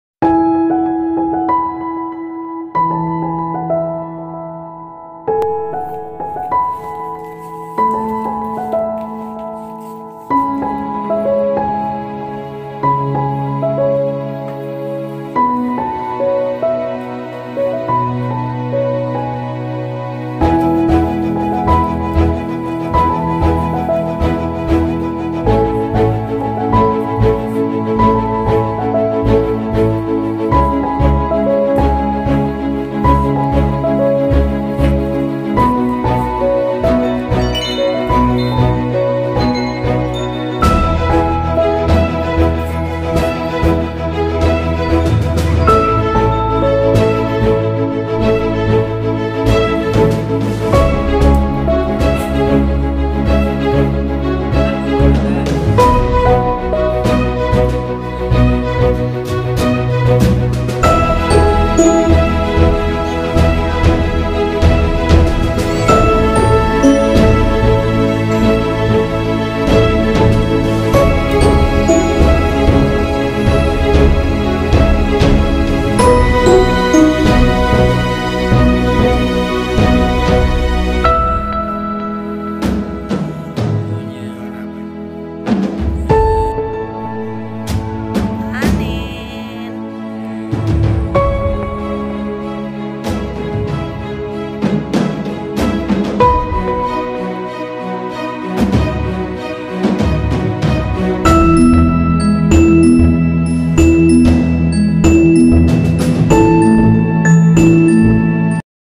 Panen hidroponik
Hari ini kakek ku melakukan panen hidroponik yang kedua dengan bayam dan kangkung.